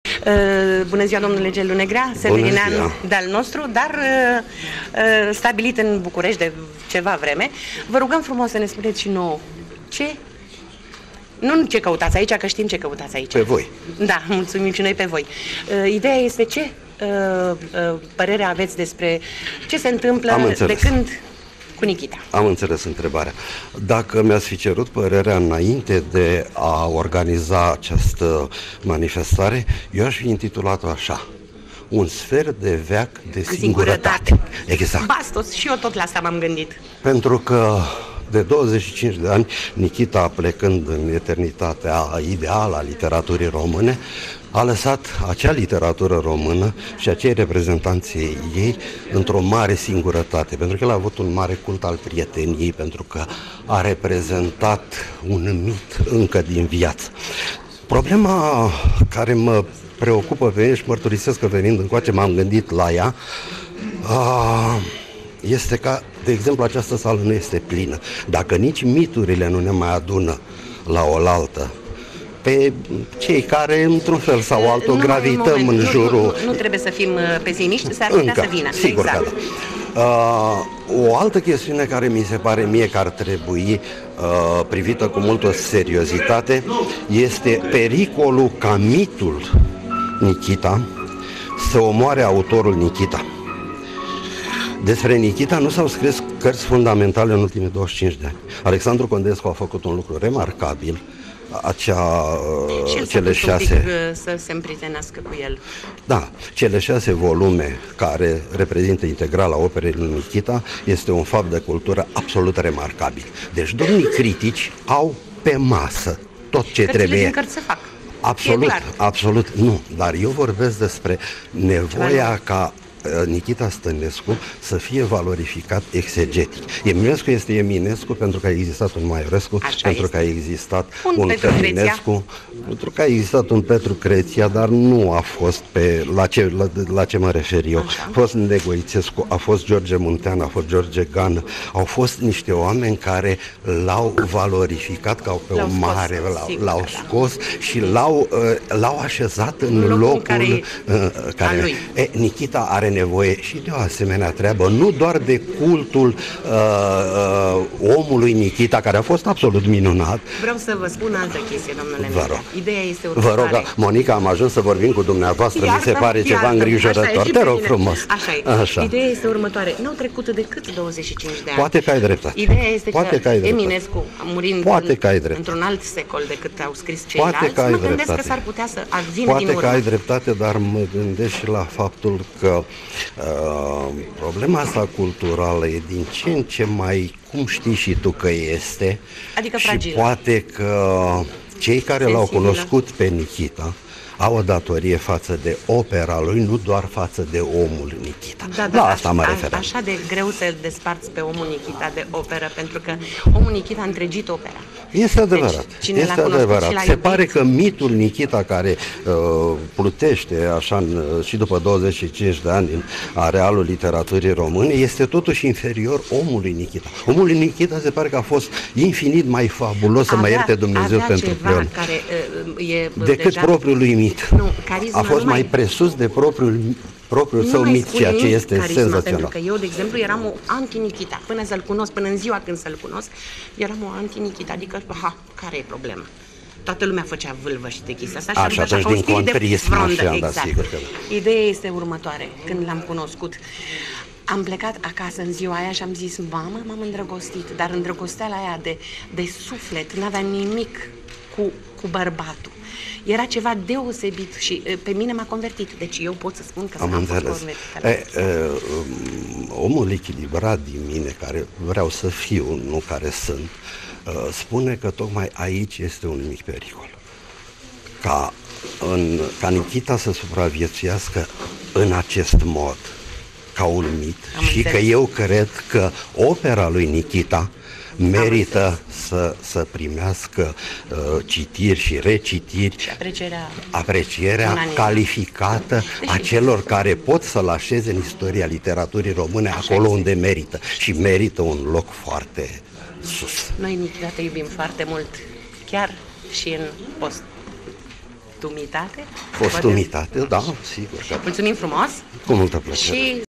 Interviu cu Gelu Negrea despre Nichita Stănescu
Interviu realizat de Monica Foamete Ciurea

NVD TV